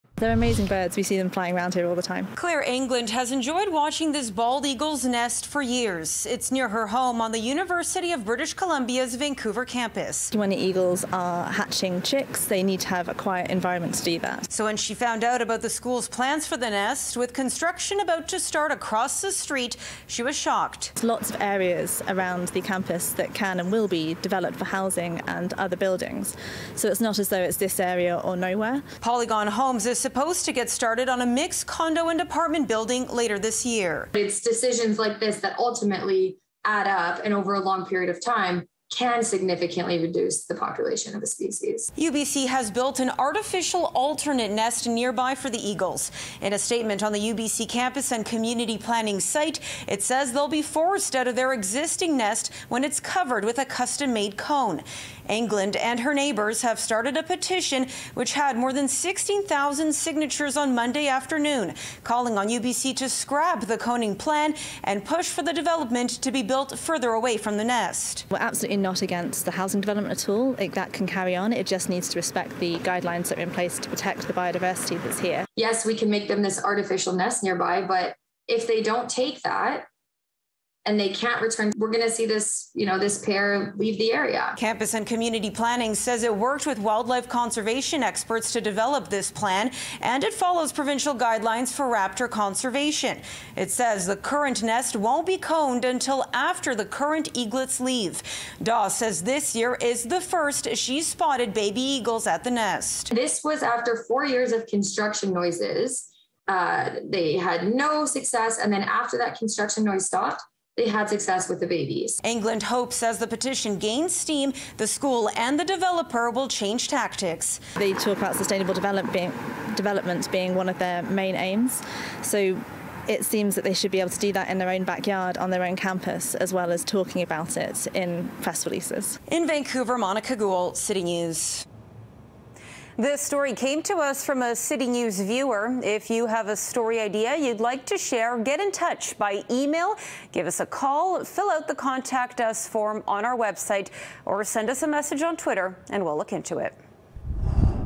Petition calls for change to UBC development near eagle’s nest
Construction on a multi-unit residential building at UBC is set to begin later this year. But Monika Gul reports a petition is calling for the development to be built further away because of the eagle’s nest across the street.